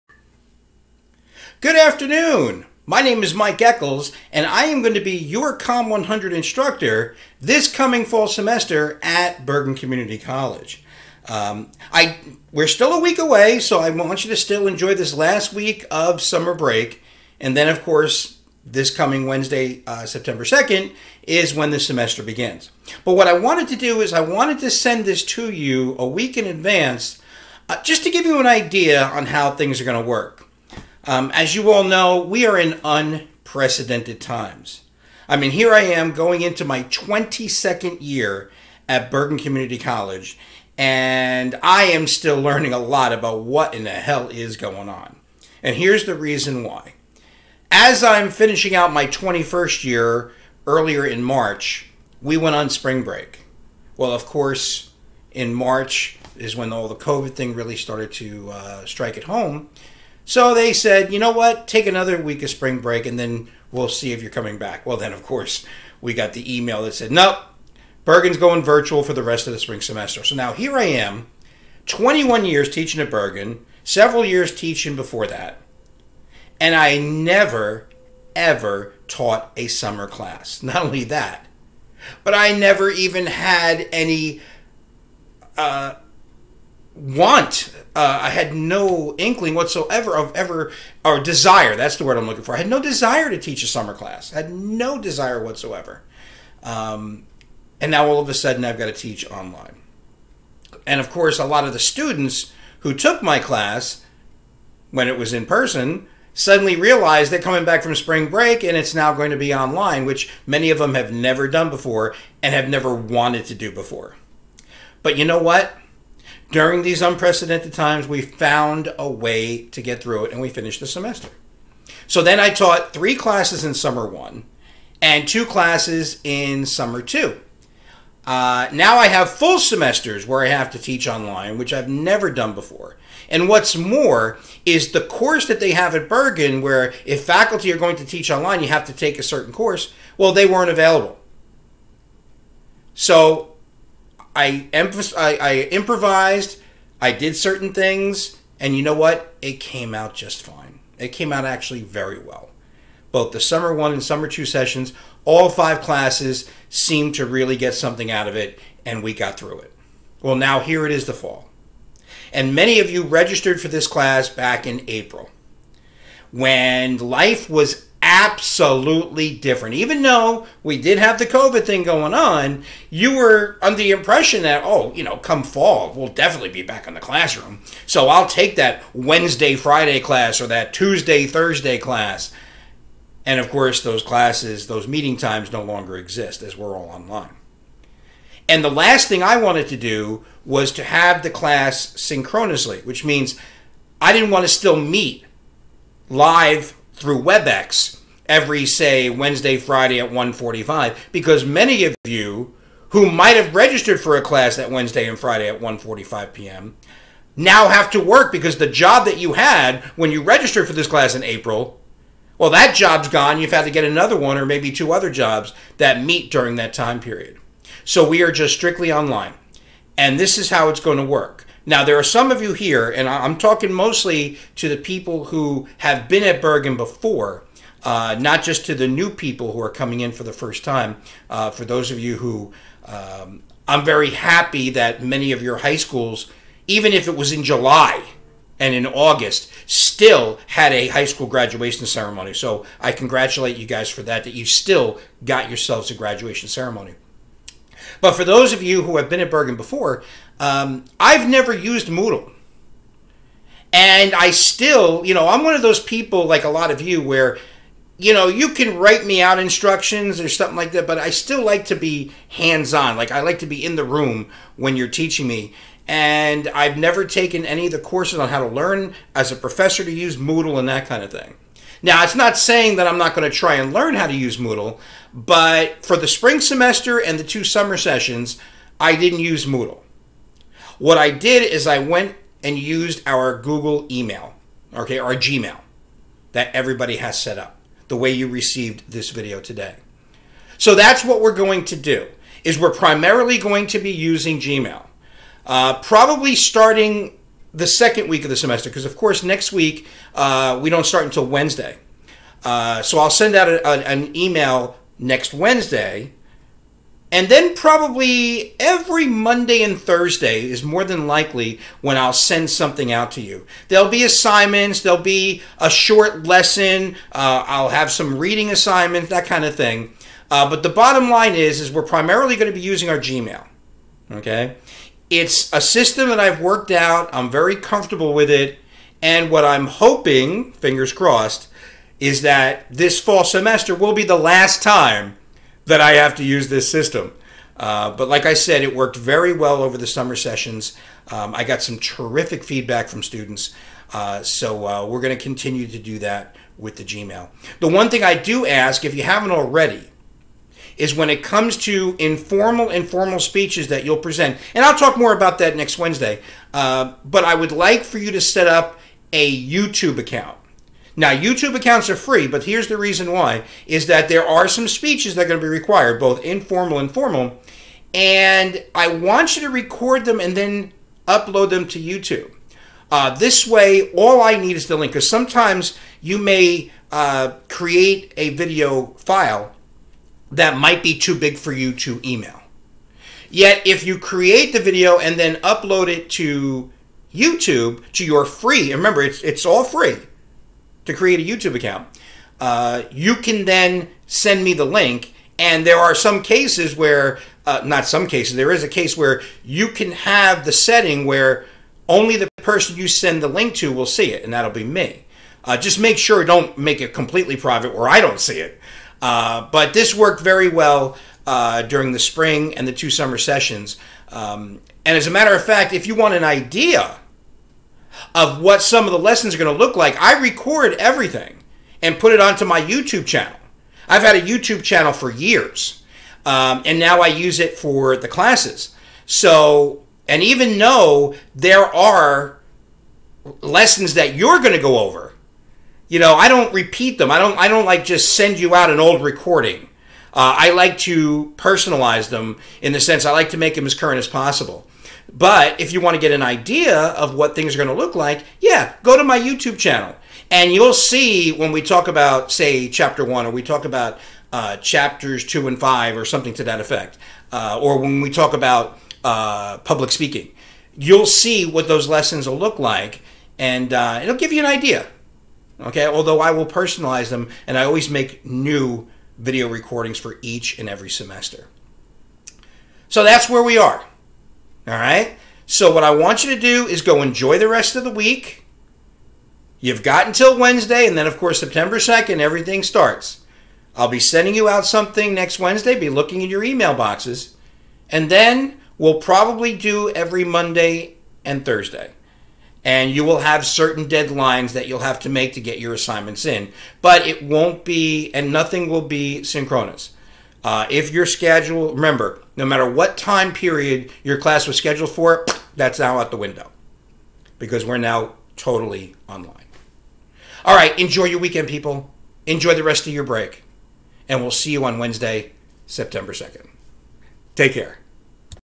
Welcome to Bergen CC Fall 2020 Com-100
Just a little bit about how the course will go!